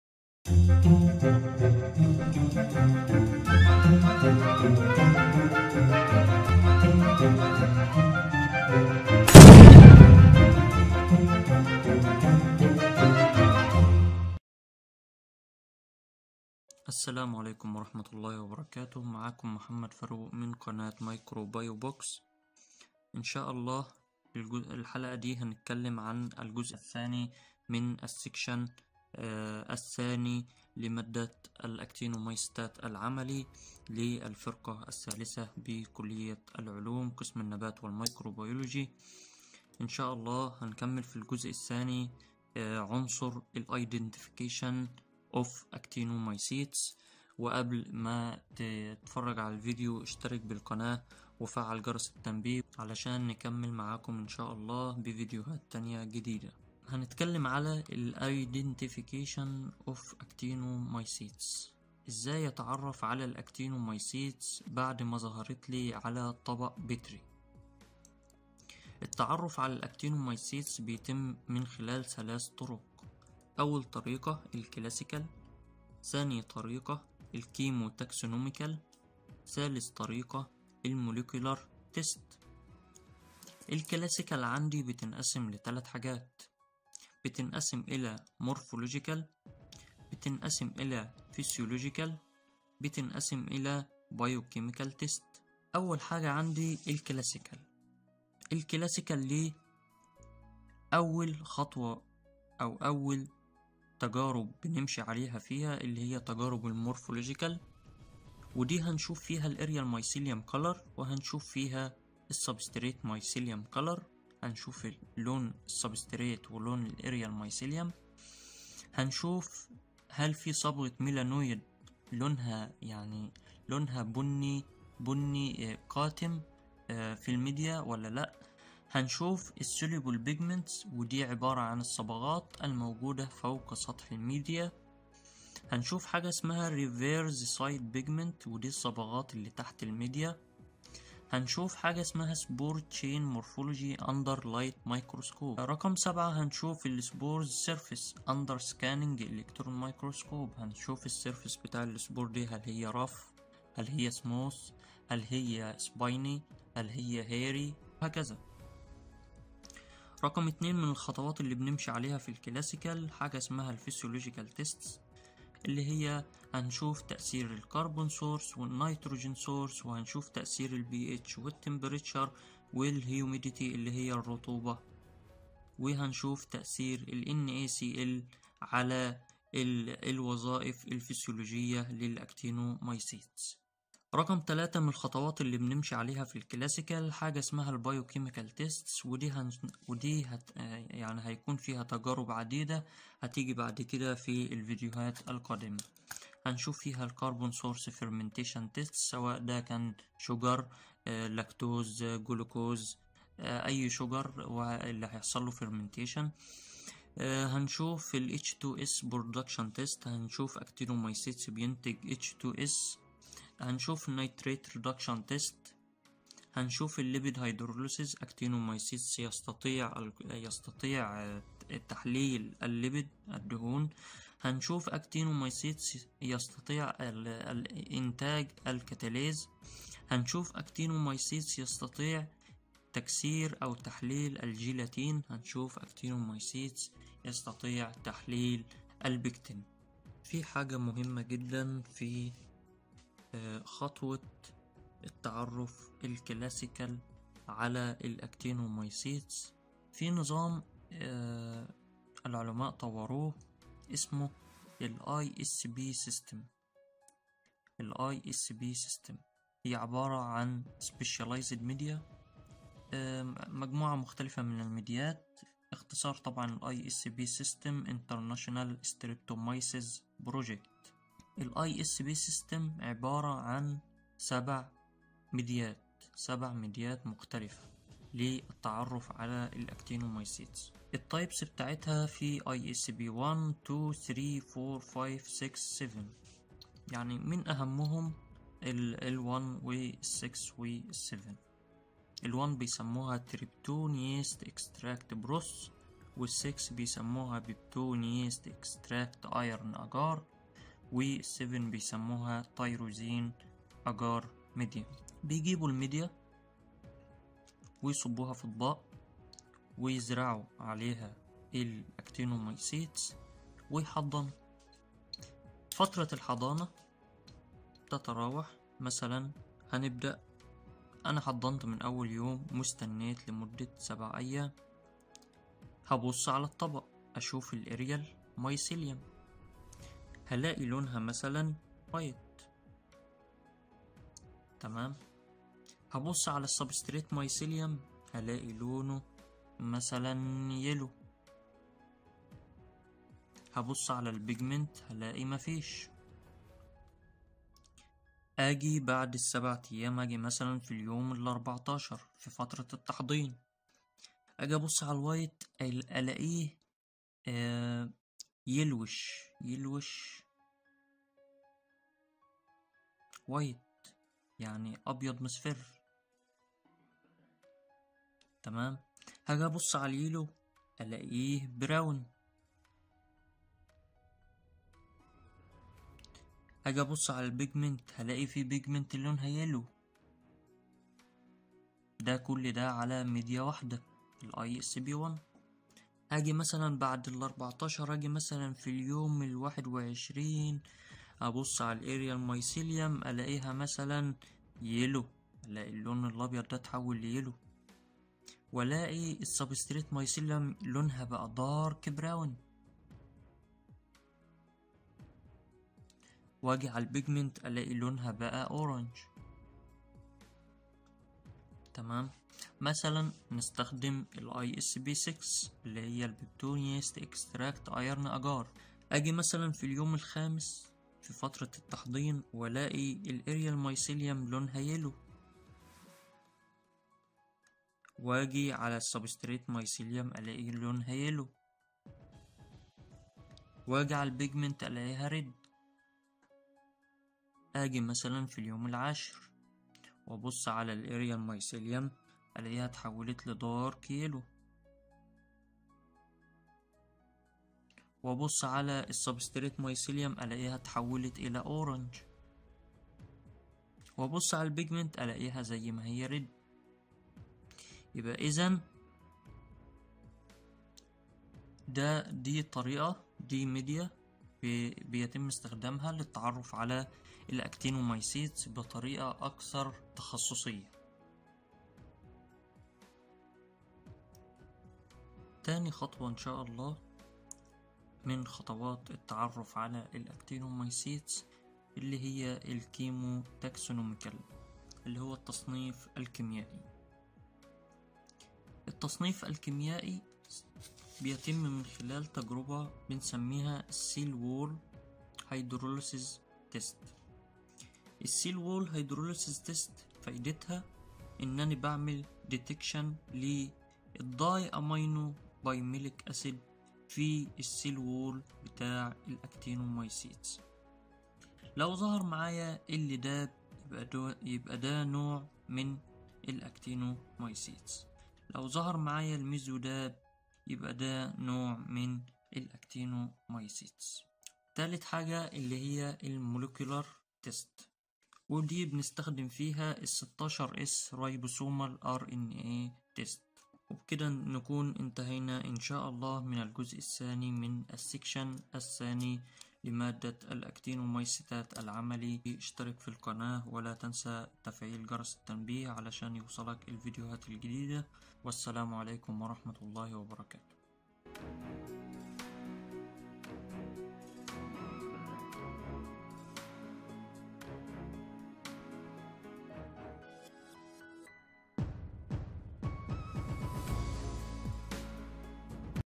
سكشن أكتينوميسيتات | الحلقة الثانية 2 | الجزء الثاني 🧫 ثالثة ميكروبيولوجي 🔬
We provide you a package of samples photos in our video to guide you to get knowledge to passing your exams.
 Actinobacteria ( Actinomycetes ) sample , faculty of science ,Alazhar university